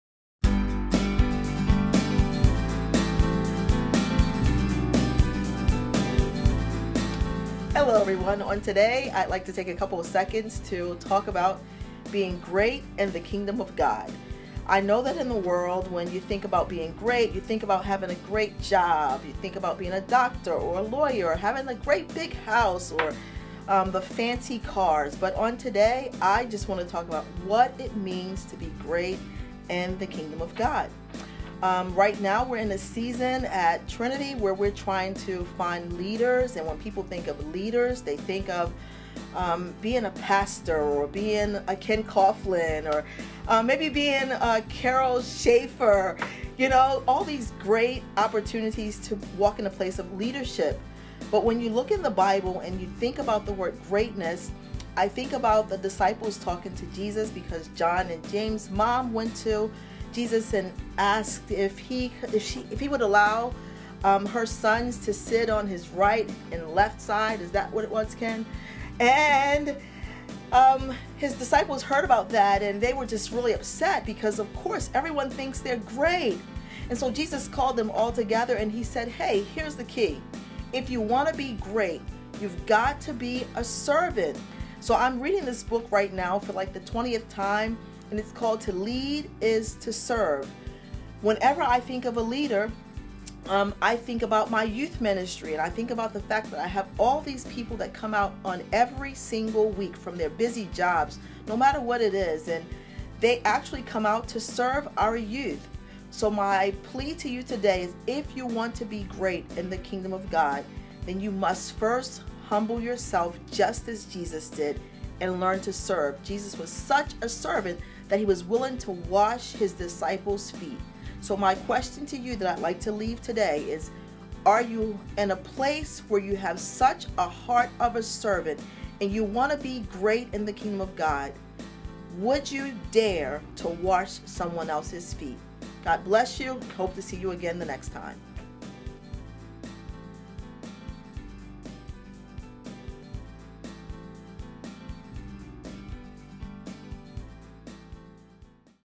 "How to Be Great" - Trinity Talk Devotions #19 (05/10/18)
Ressee Turner, Youth Leader